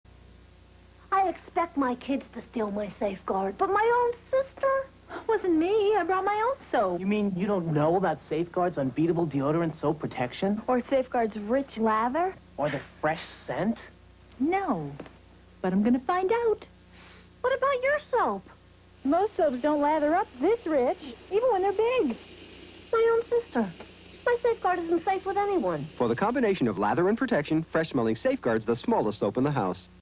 1981 Safeguard Soap "The Smallest Soap in the House" TV Commercial
I have used monetization to cover costs of new content acquisition. Its not much but it does help.

The commercial was originally aired in November 14, 1981. It was shown during NBC News coverage of the STS2 Space Shuttle Landing on Channel 4 KNBC.